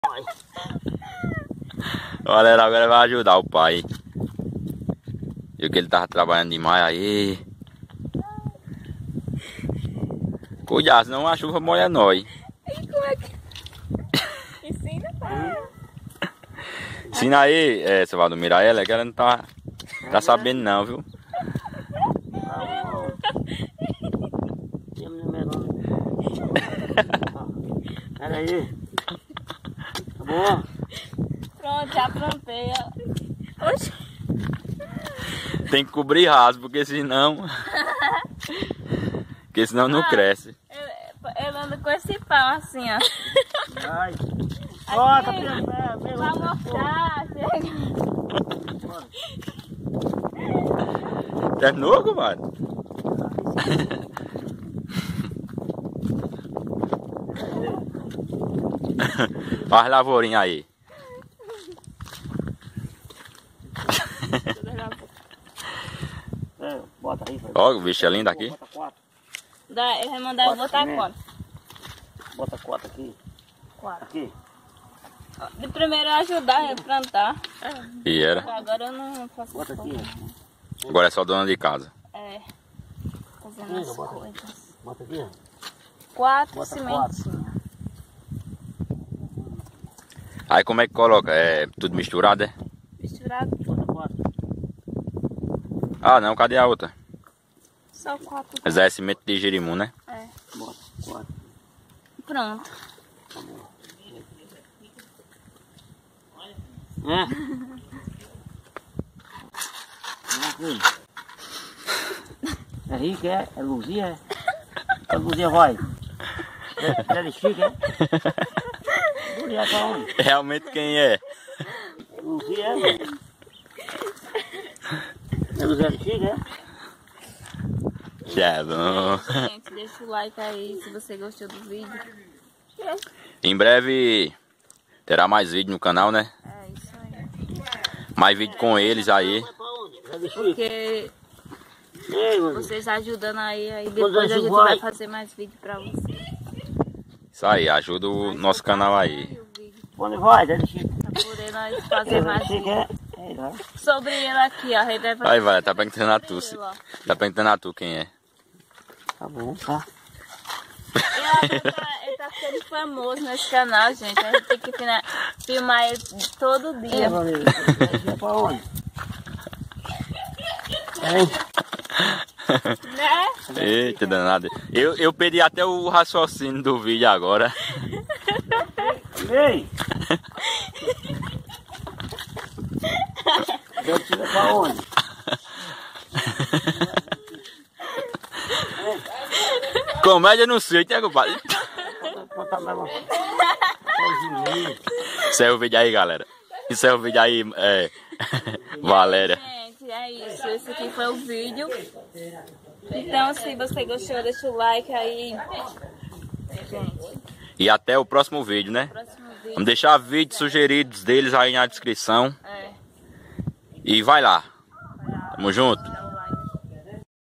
VALÉRIA AJUDOU SEU PAI VALDOMIRO A PLANTAR - COMÉDIA NO SÍTIO CONCEIÇÃO DE CACIMBA DE DENTRO PB
Vídeo gravado no Sítio Conceição Cacimba de Dentro PB.